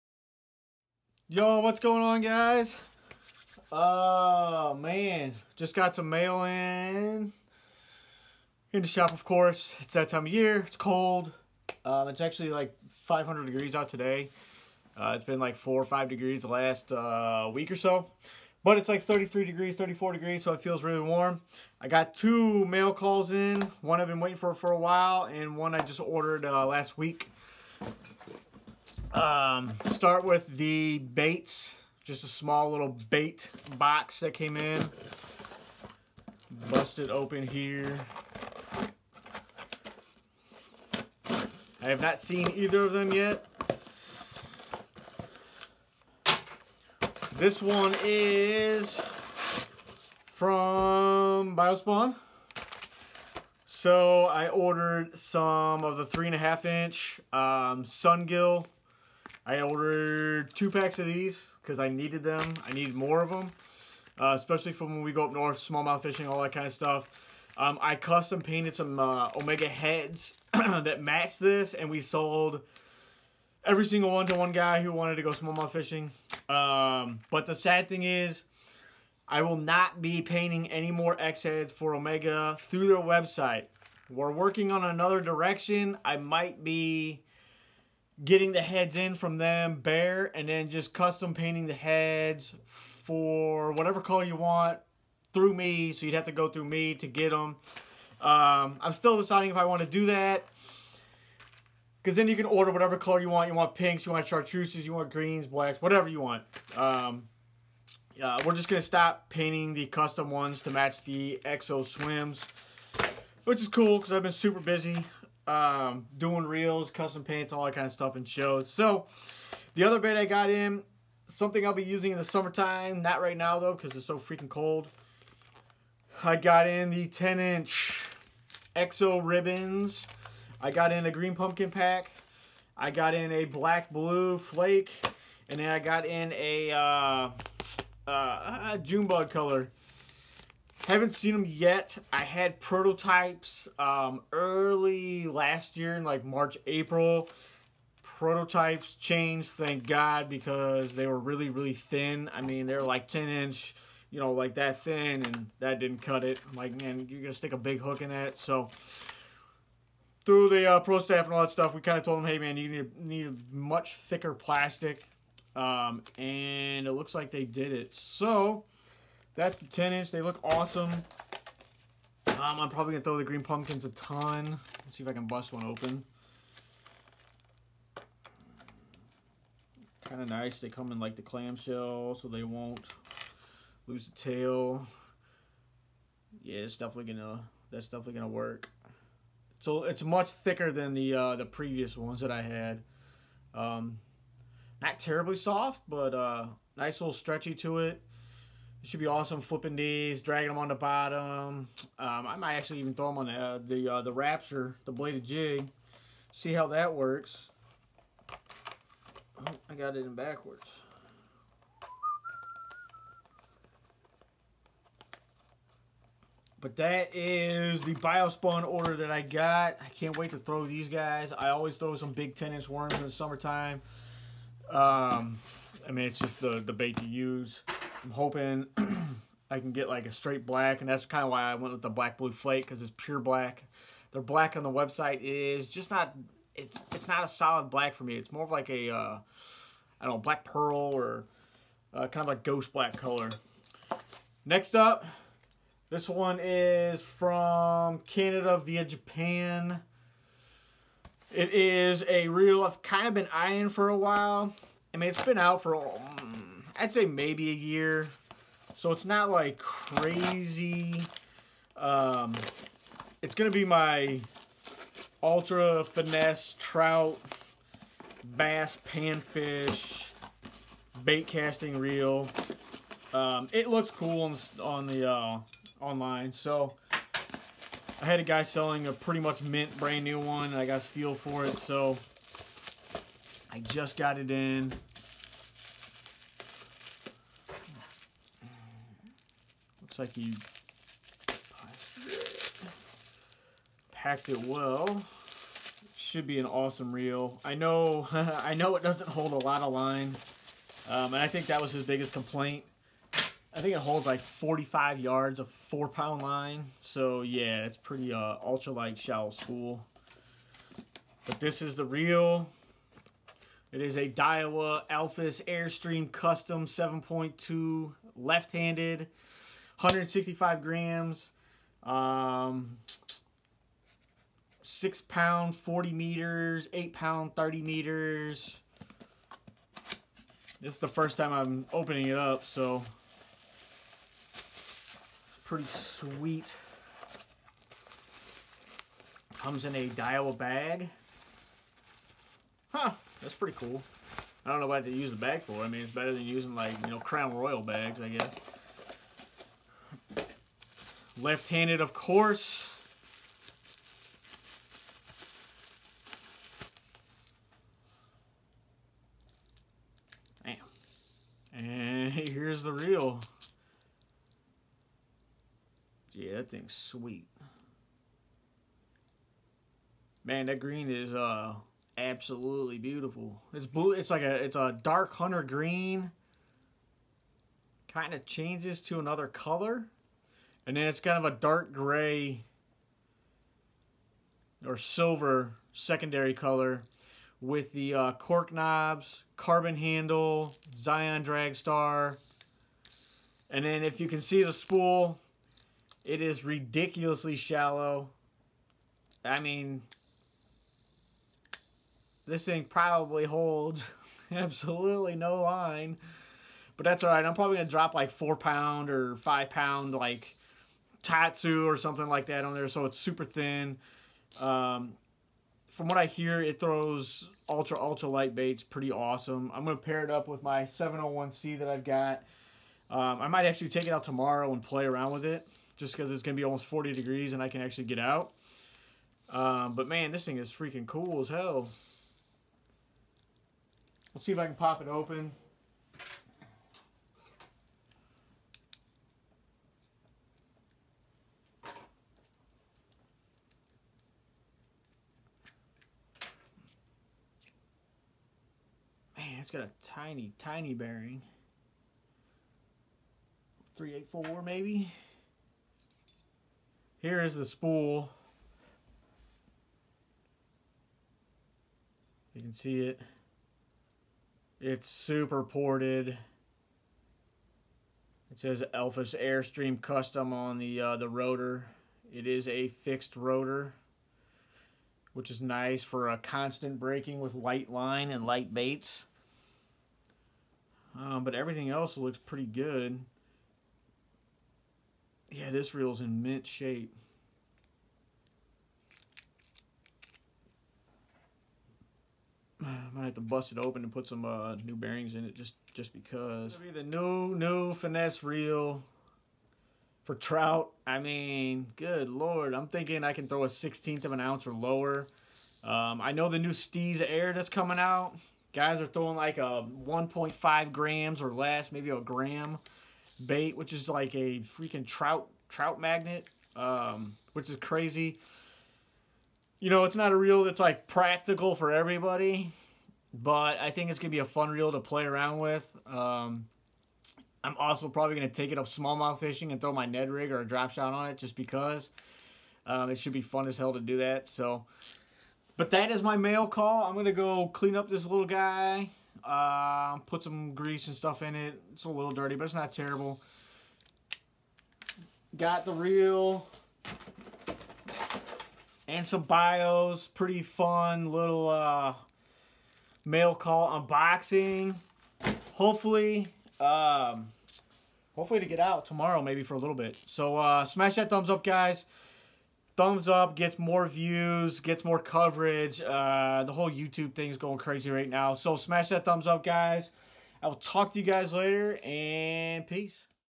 Double un Boxing! Mega finesse BC and Baits...
Daiwa Alphas Air Stream Custom

5.6ozs
7.2.1 gears

carbon handle

BIOSPawn 10" worms and Exo swims